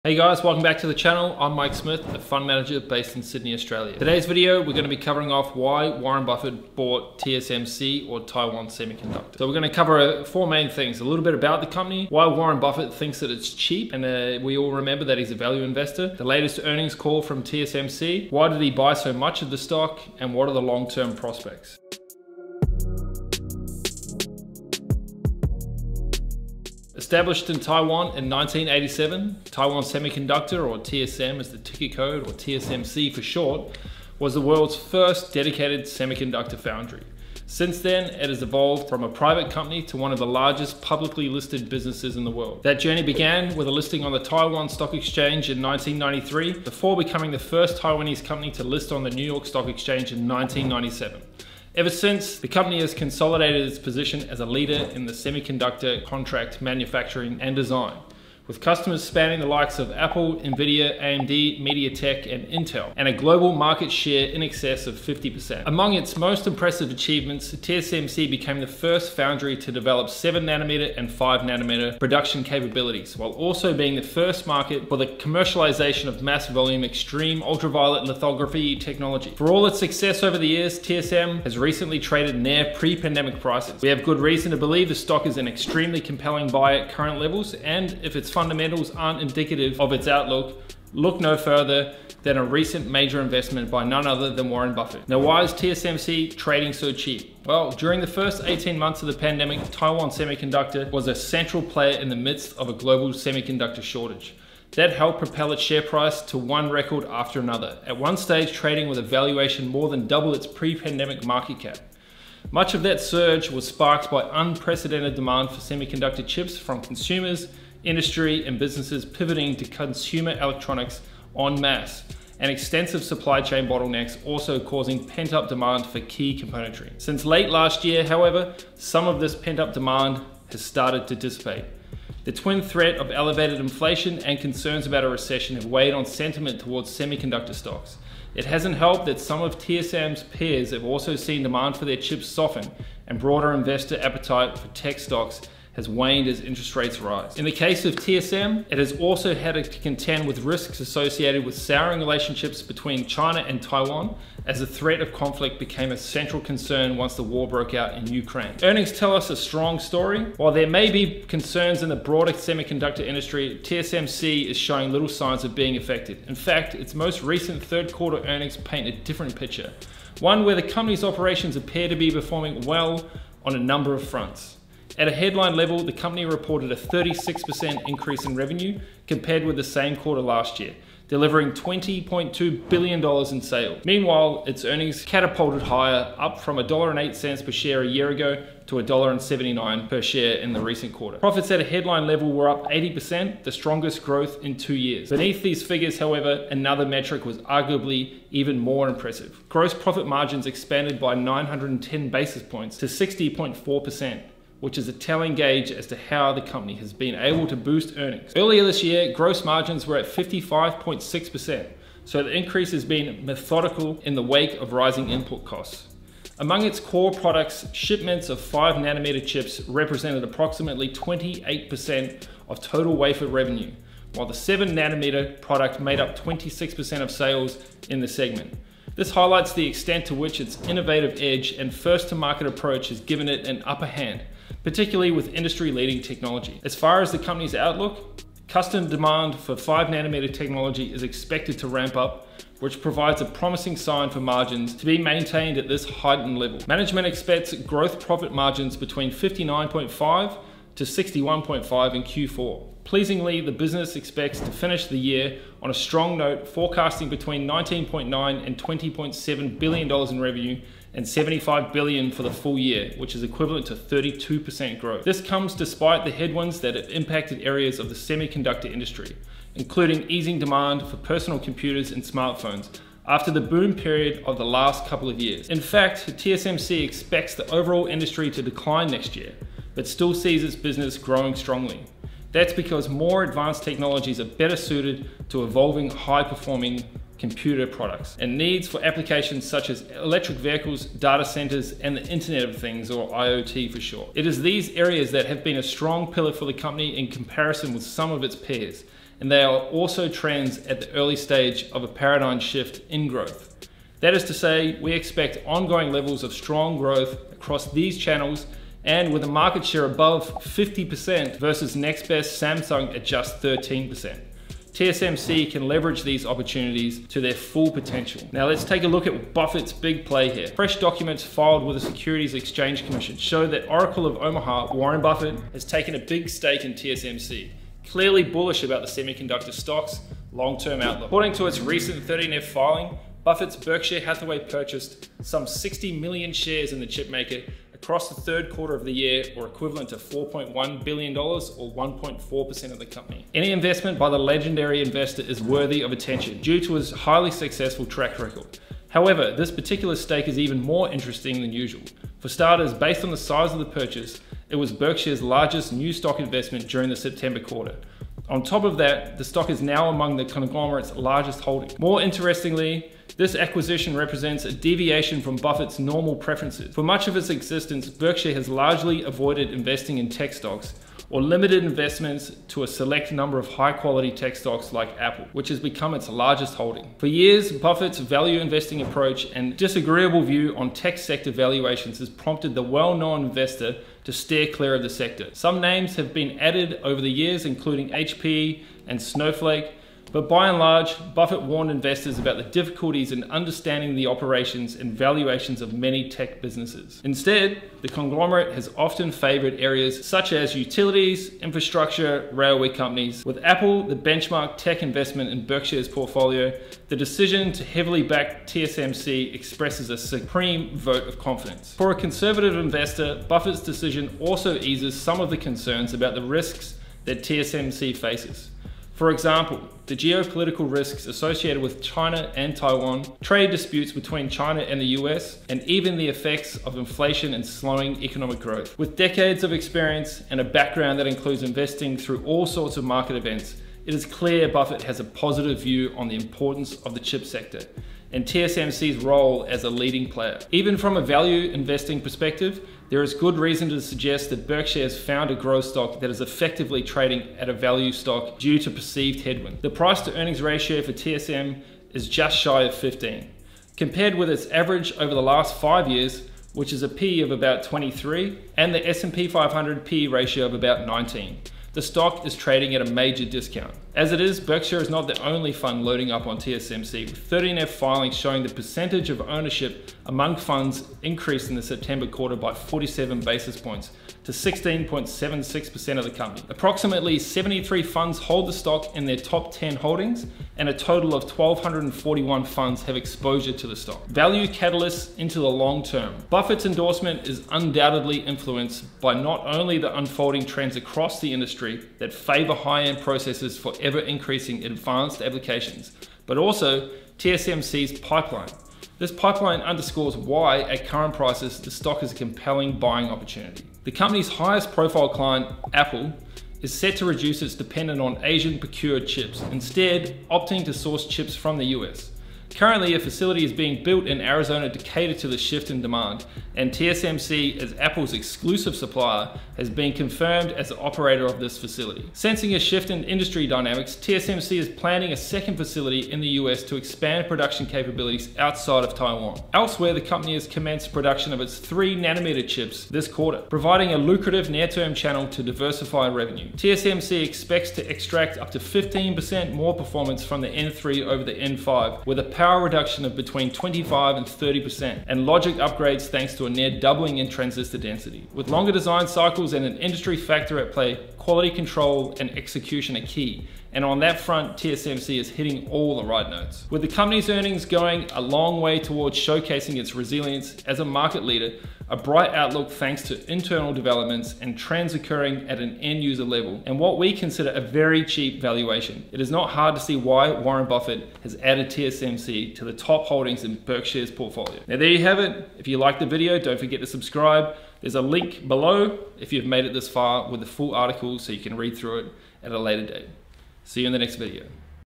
Warren Buffett's HUGE Bet on TSMC (TSM) | BRKB AAPL | Why Did Buffett Buy $4.1B Of TSMC
Warren Buffett just made a huge investment in TSMC. In this video, I take a look at why he made such a large bet.